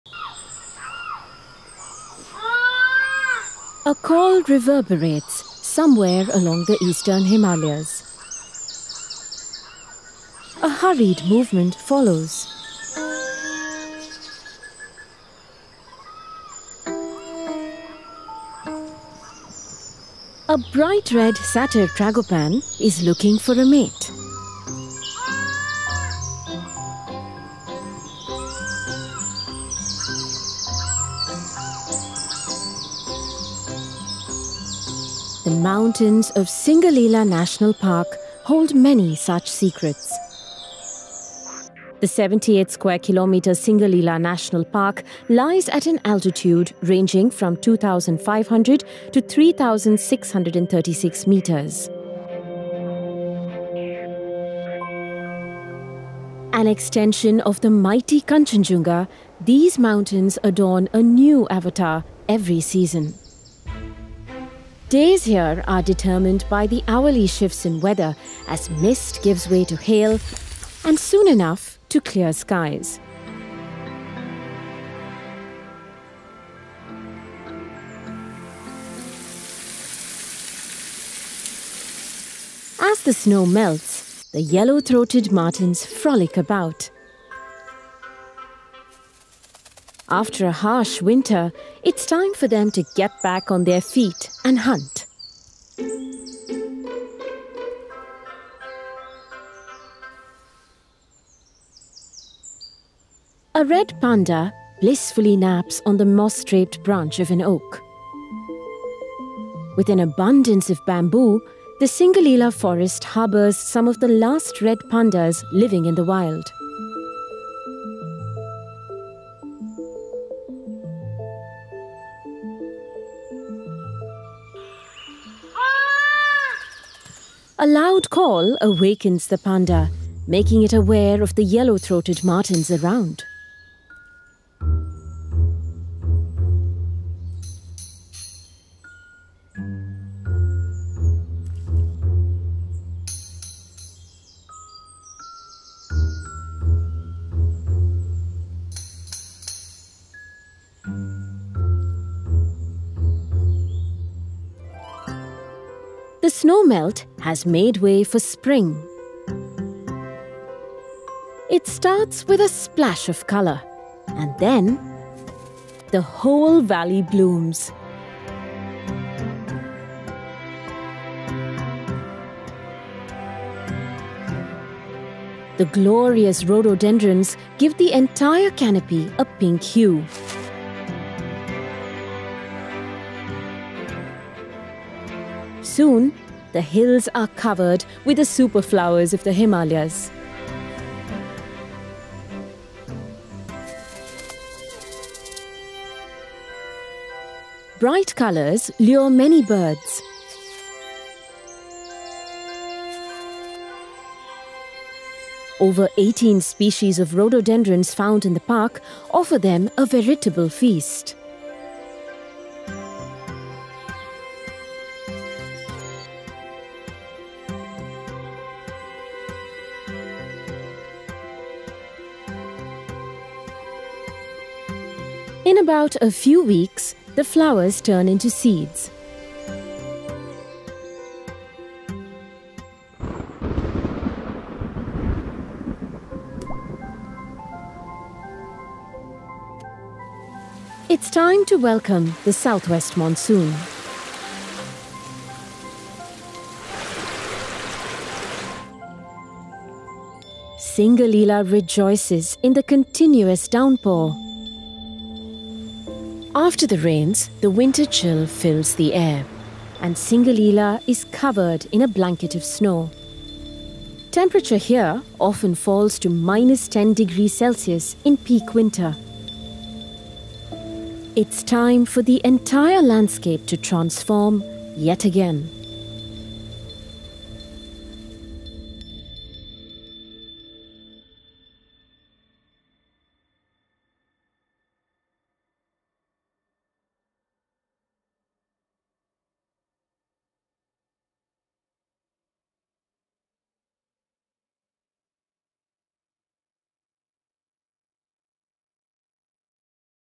The Misty Mountains of Singalila
Located along the Indo-Nepal border, the high-altitude mountains of Singalila National Park safeguard many secrets. With every season, the landscape transforms, donning a new avatar and turning into a haven for both — its resident creatures and visiting wild guests.